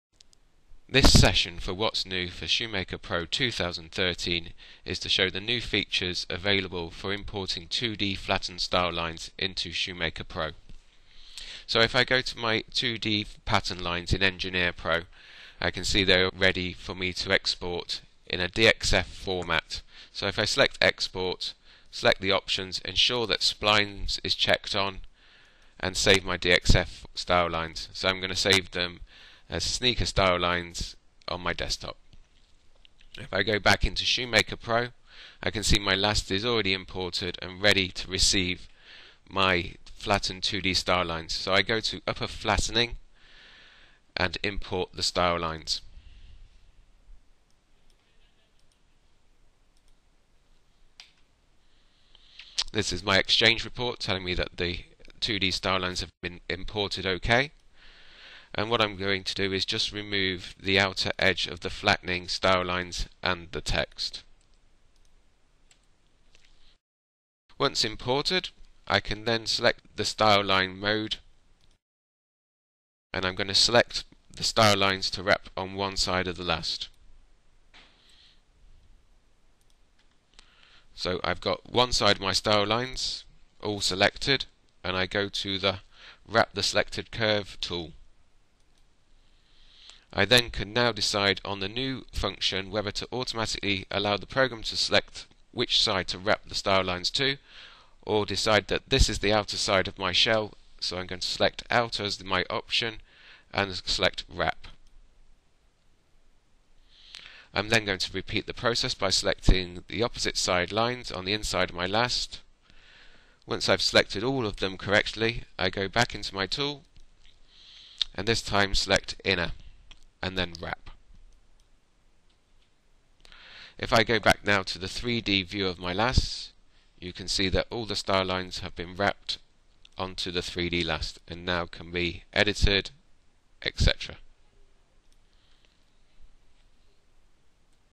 ShoeMaker 2013 R1 - Importing 2D Stylelines
See how easy it is to import 2D style lines into 3D design software, Delcam CRISPIN ShoeMaker Pro.

Delcam CRISPIN ShoeMaker provides sophisticated yet intuitive design tools so you can produce realistic 3D digital models. This allows you to approve designs in less time and reduces the need for expensive physical prototypes.